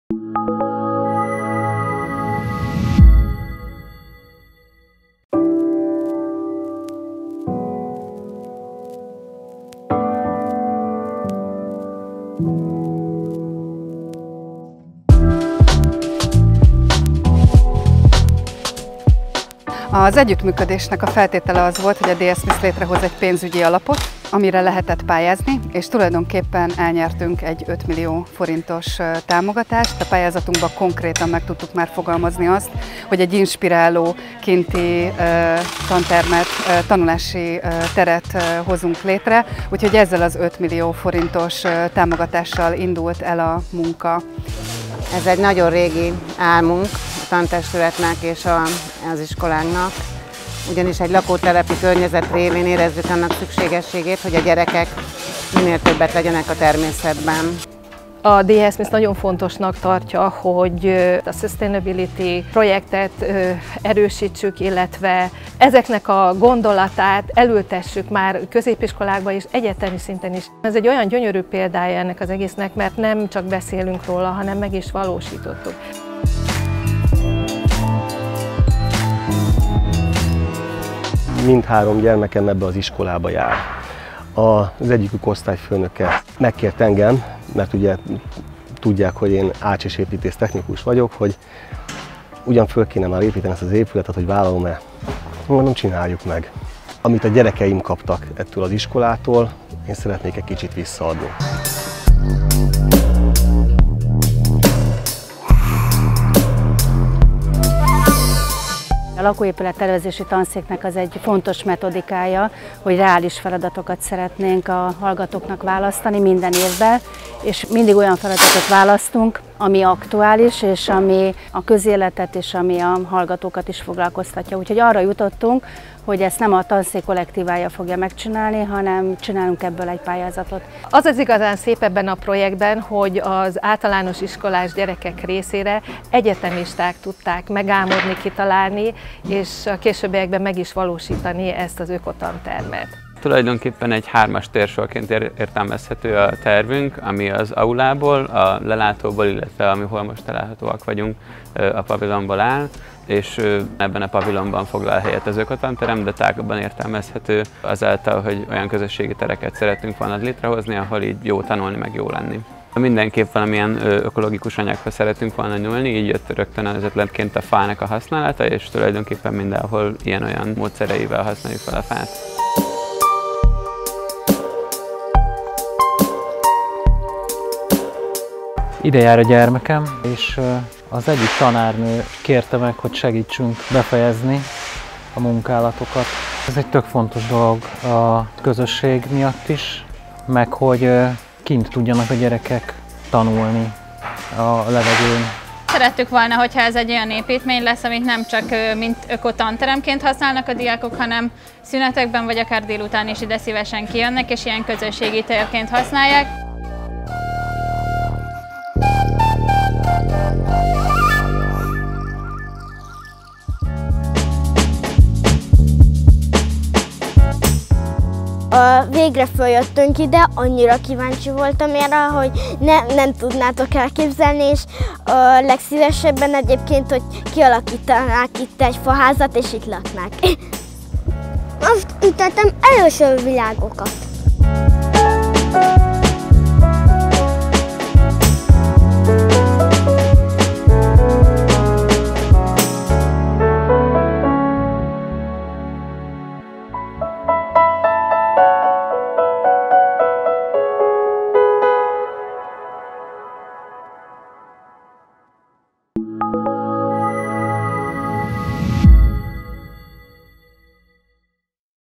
DS Smith Eco-classroom (Hungarian)
DS Smith supports construction of Eco-classroom in Hungary.
DS Smith released a design competition with technically patronage of Residential Building Design, Faculty of Architect of BME (Budapest University of Technology and Economics Department). The competition called for architecture students in Hungary to design a classroom that both motivates students and propagates sustainable approach.